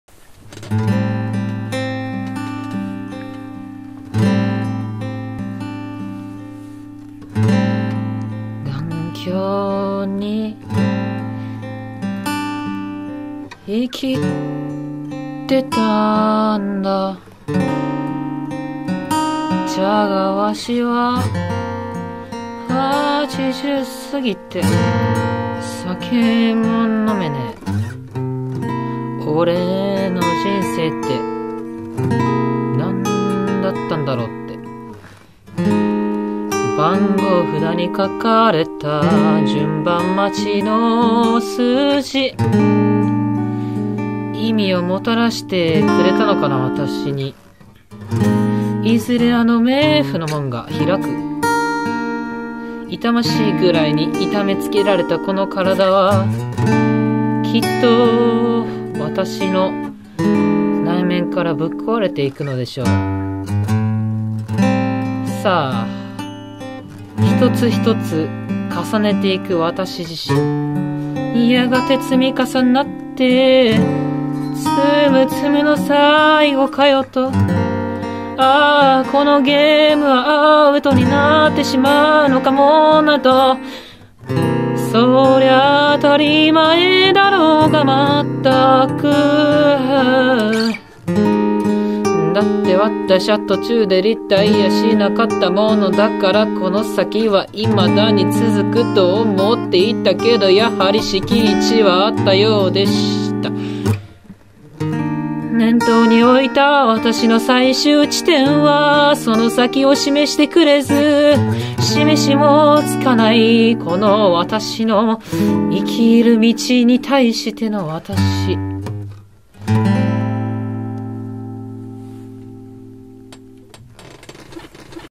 生きる道に対しての私
頑強に
生きてたんだ
じゃが
わしは
80過ぎて
酒も飲めねえ
俺の人生って
何だったん
だろうって
番号札に
書かれた
順番待ちの数字
意味を
もたらして
くれたのかな
私に
いずれあの
冥府の門が開く
痛ましいぐらいに
痛めつけられた
この体は
きっと
私の
内面から
ぶっ壊れて
いくのでしょう
さあ
一つ一つ
重ねていく
私自身
やがて
積み重なって
ツムツムの
最後かよと
ああ
このゲームは
アウトになって
しまうのかもなど
そりゃ
当たり前だろが
全く
だって
わたしゃ
途中で
リタイア
しなかった
ものだから
この先は
未だに
続くと
思っていたけど
やはり
閾値は
あったようでした
念頭に置いた
私の最終地点は
その先を
示してくれず
示しもつかない
この私の
生きる道に
対しての
私